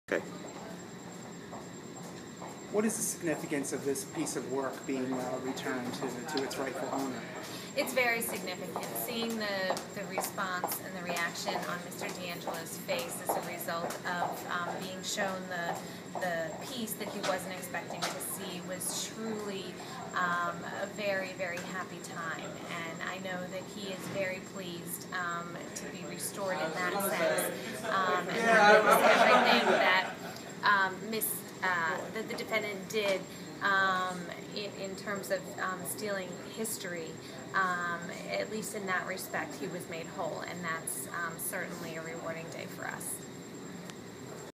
treasure returned
Special Montco Assistant District Attorney Devon Turner talks about the importance of returning "family treasure" to elderly burglary victim.